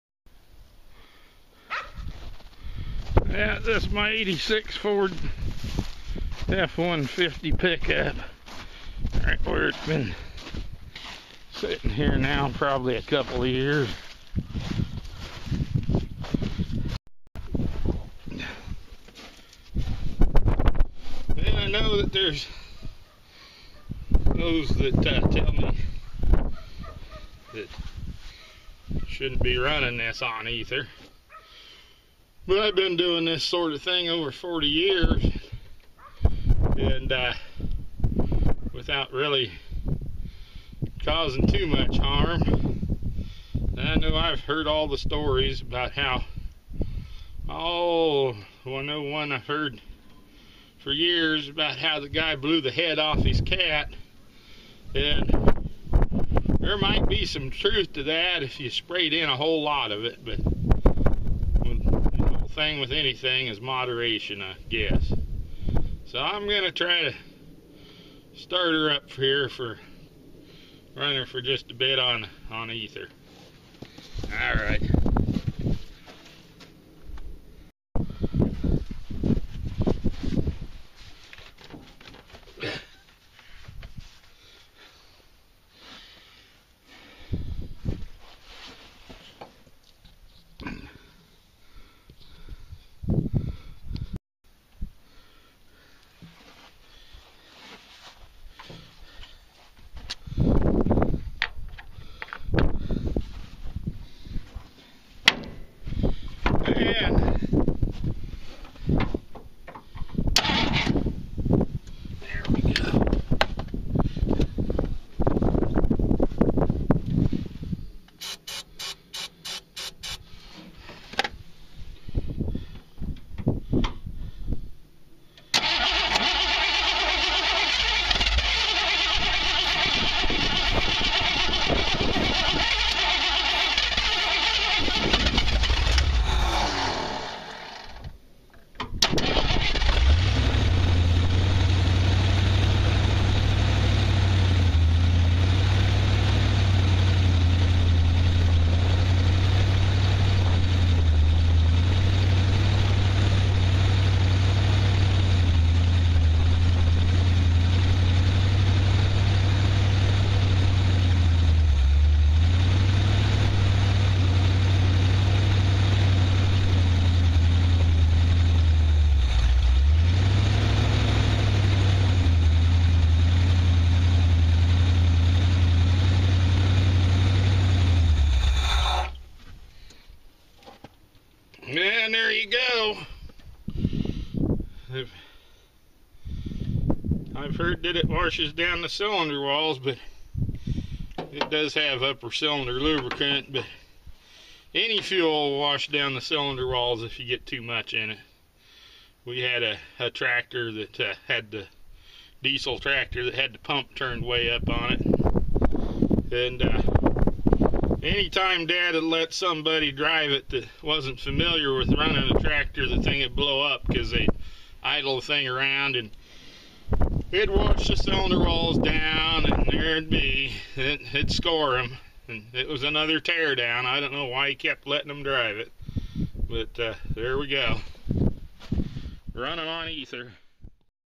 86 Ford F150 4X4 Runs on Ether
Running a 1986 Ford F150 that has been sitting for years on starting fluid.